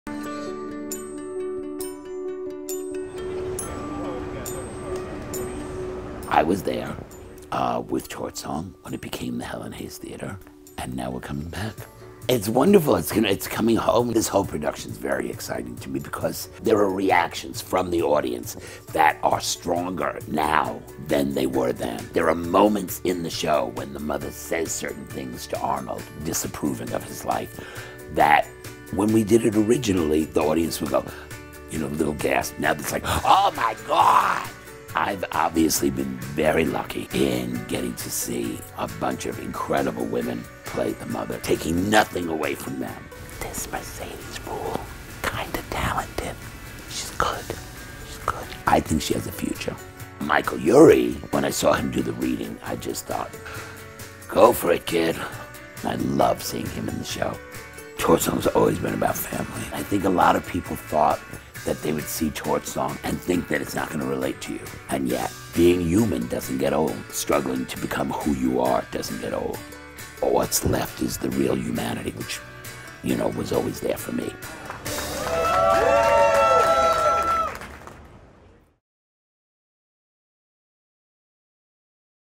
Harvey Fierstein Carries The Torch To Broadway
Harvey Fierstein carries the torch to Broadway this fall.

Harvey Fierstein’s Torch Song starring Michael Urie and Mercedes Ruehl. Directed by Moisés Kaufman.